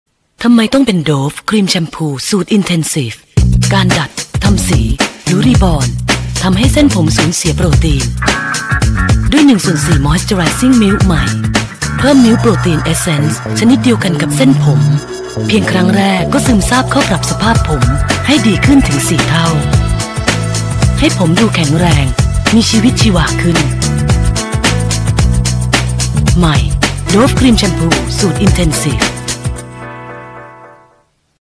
Dove Moisture Milk Intensive Shampoo launch 30s - Thailand, 2004
โฆษณา โดฟ ครีมแชมพู สูตรอินเทนซีฟ ปีพ.ศ. 2547